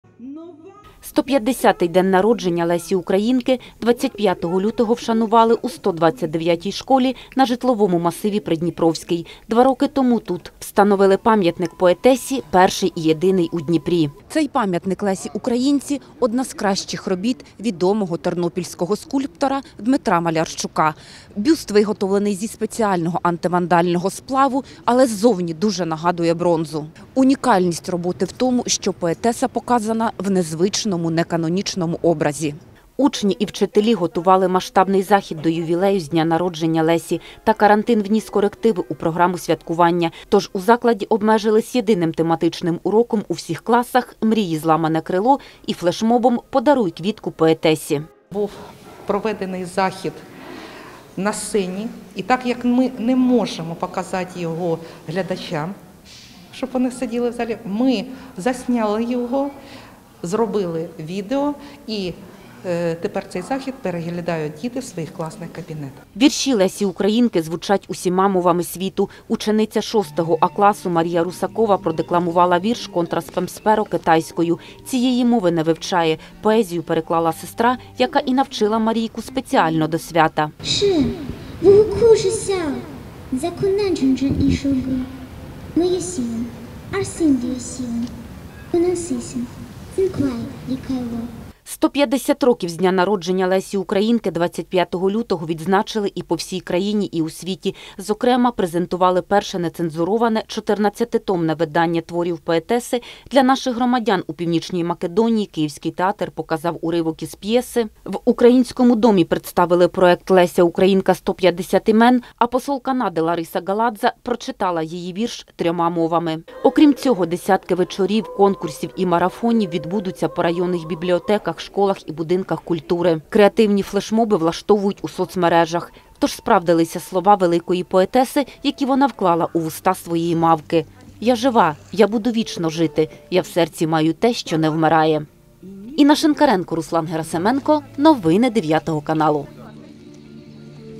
Єдиний у Дніпрі пам’ятник Лесі Українці встановлений на подвір’ї 129-ї школи
25 лютого — 150 років від дня народження Лесі Українки.